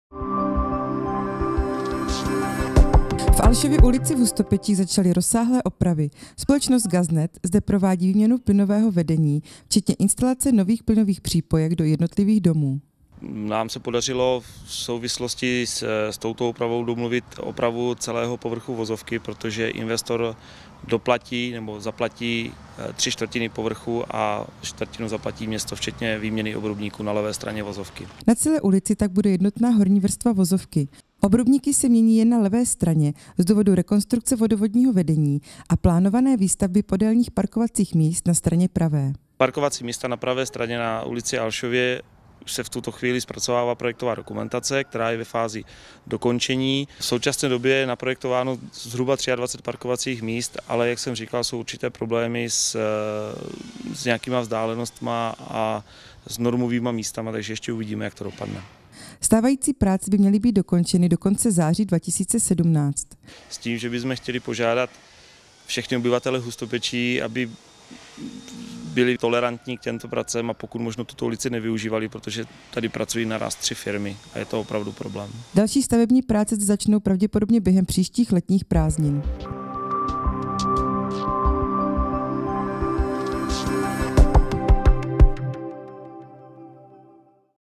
Stavební práce v Alšově ulici
V Alšově ulici právě probíhají stavební práce, na kterých se podílí tři firmy. Dojde zde i k opravě povrchu vozovky.